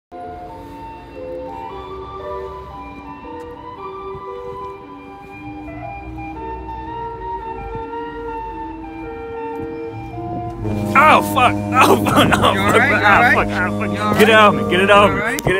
Doogie going for a lil walk
A lot of people don’t know this about me, but me personally, I like taking walks